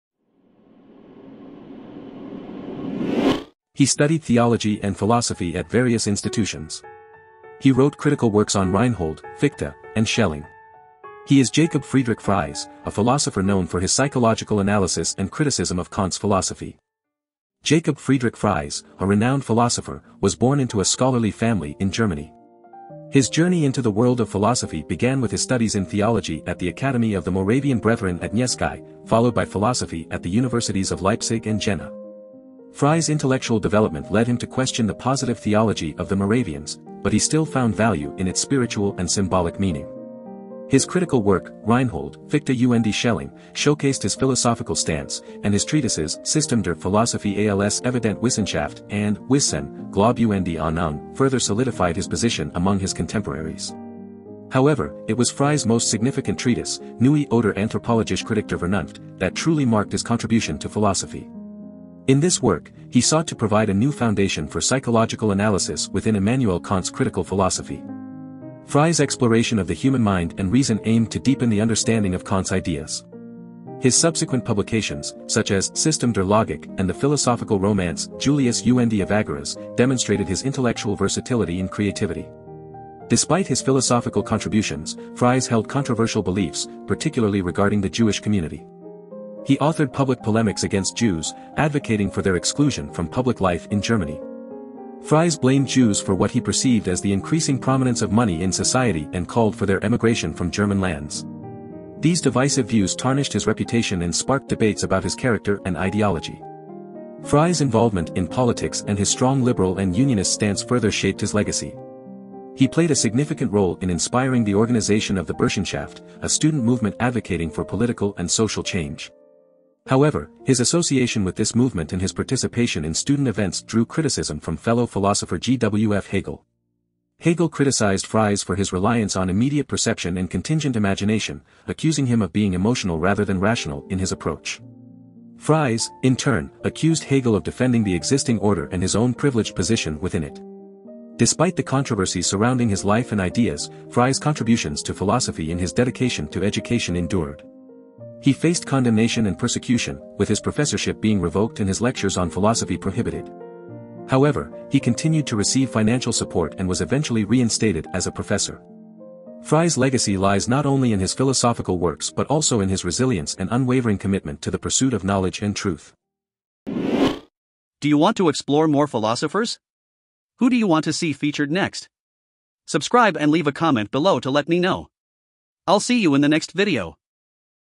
Jakob Friedrich Fries: Unveiling Kant's Critic｜Philosopher Biography
Jakob Friedrich Fries, a renowned philosopher, was born into a scholarly family in Germany. His ……

8429